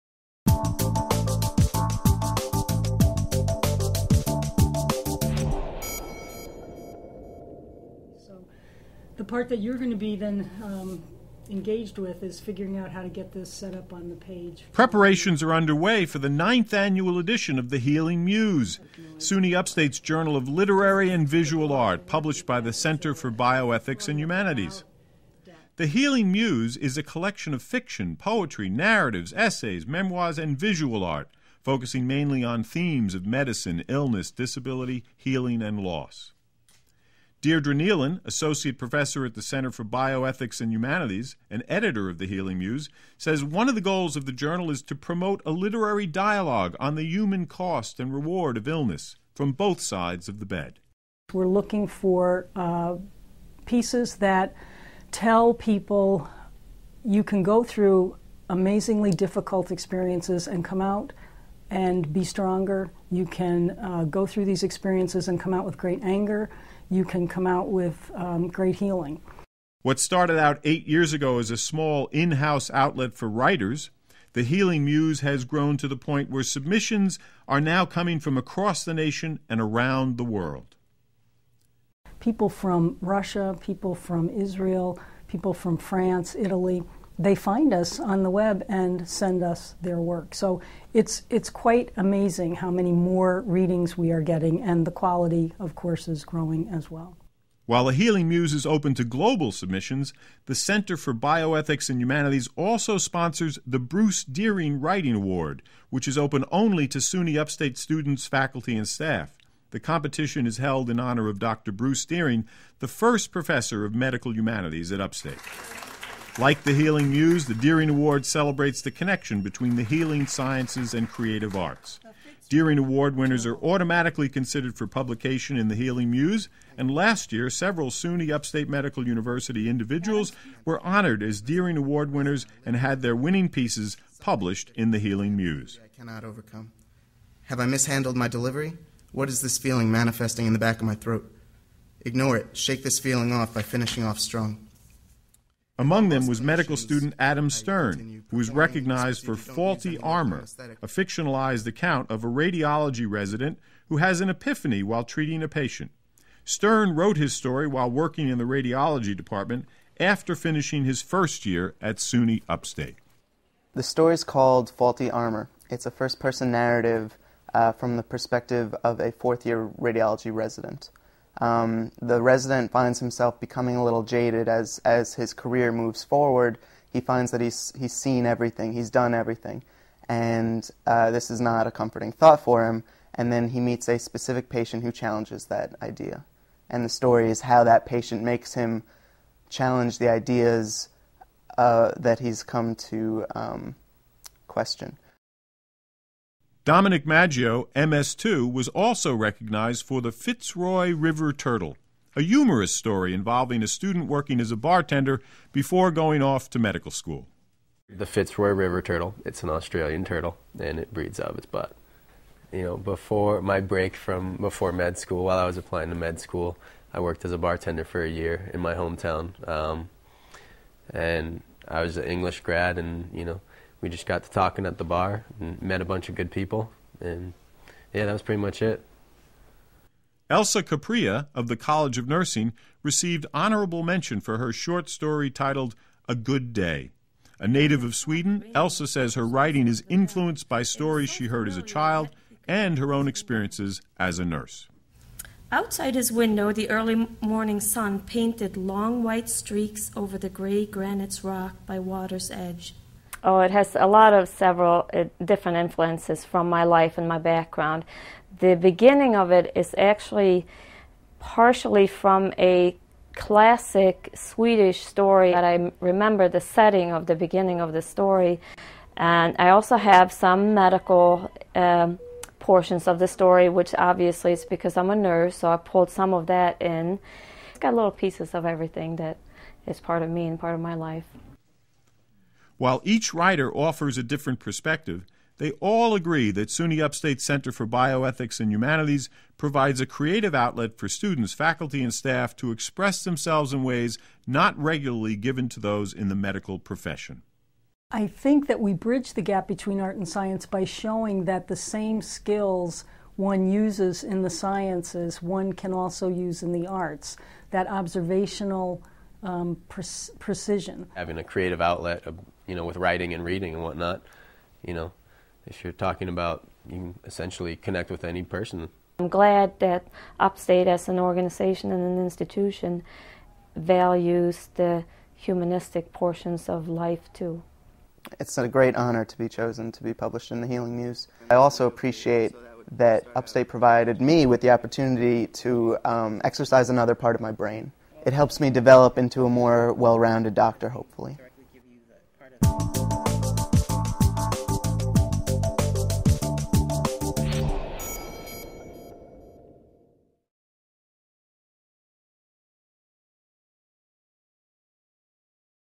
Healthlink Extra-The Healing Muse
In this edition of Upstate Medical University's Healthlink video series, we feature the Center for Bioethics and Humanities at Upstate Medical University in Syracuse, NY. This segment highlights The Bruce Dearing Writing Awards and "The Healing Muse", Upstate's literary and visual arts journal, published annually by the Center for Bioethics and Humanities.